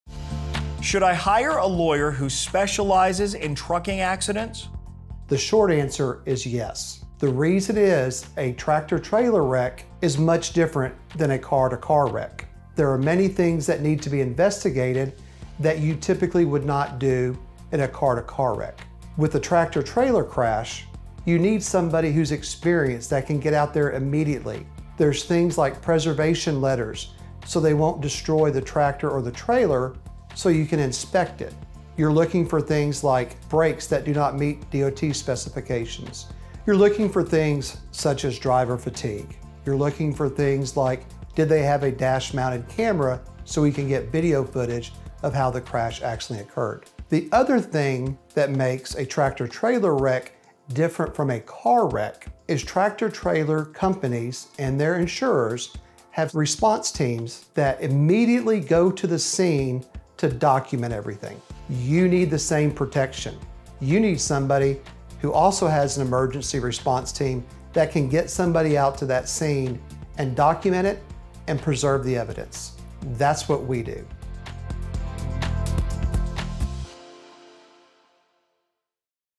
Should I Hire a Lawyer Who Specializes in Trucking Accidents? | Jay Murray Personal Injury Lawyers
Should I hire a lawyer who specializes in trucking accidents? The short answer is yes.

Why? A tractor-trailer accident differs significantly from a typical car accident. Many aspects must be investigated that usually wouldn’t be required in a car crash.

You need an experienced damage assessor who can visit the accident scene quickly after the collision. You also need preservation letters so you can inspect the truck before they destroy the tractor or trailer. An investigator will look for things like faulty brakes, driver fatigue, and dash camera footage availability.

Trucking companies and their insurers have response teams that immediately visit the scene to document everything. You need someone on your side – such as a dedicated Dallas truck accident attorney – to do the same for you.

If you were involved in a collision with a tractor-trailer in Dallas, TX, contact the Jay Murray Law Group for immediate assistance.